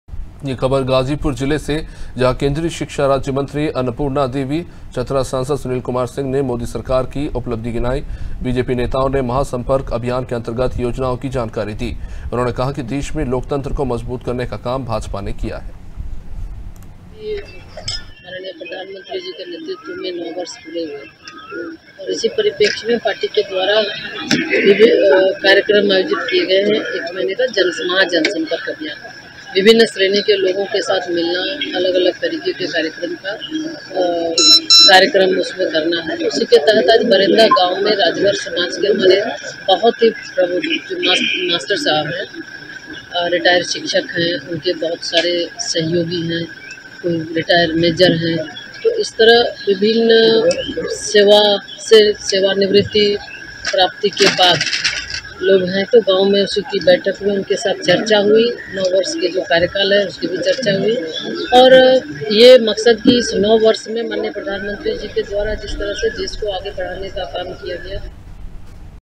Ghazipur : राज्य मंत्री अन्नपूर्णा देवी का बयान, विदेश में मोदी ने बढ़ाया देश का मान || Anaadi Tv
खबर गाजीपुर जिले से है .. जहां केंद्रीय शिक्षा राज्य मंत्री अन्नपूर्णा देवी, चतरा सांसद सुनील कुमार सिंह ने मोदी सरकार की उपलब्धि गिनाई है..बीजेपी नेताओं ने महासंपर्क अभियान के अंतर्गत योजनाओं की जानकारी दी है.उन्होने कहा कि देश में लोकतंत्र को मजबूत करने का काम भाजपा ने किया है। विदेशों में देश का सम्मान बढ़ा है। आज भारत की क्षमता को पूरा विश्व महसूस कर रहा है...

COMING SOON
 DEN, AIRTEL